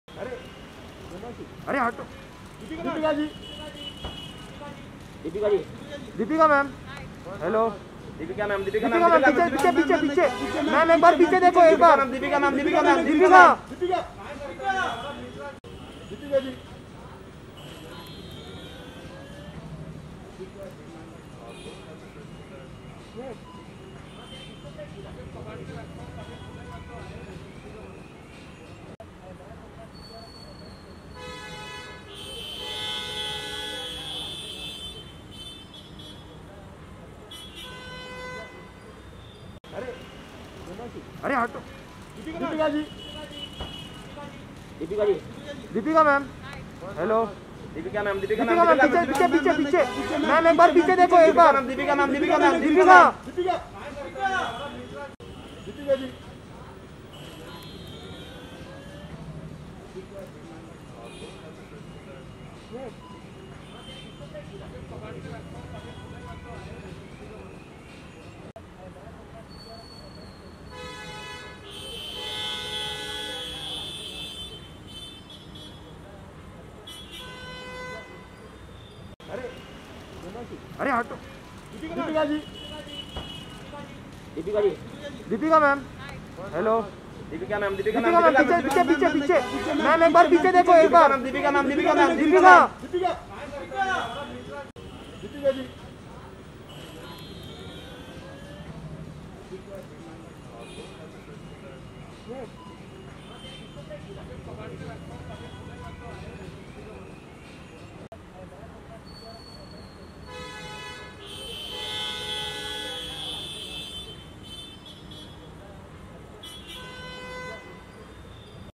Deepika Padukone Snapped At Foodhall Bandra
Deepika Padukone The Diva Of Bollywood Spotted At Foodhall Bandra Mumbai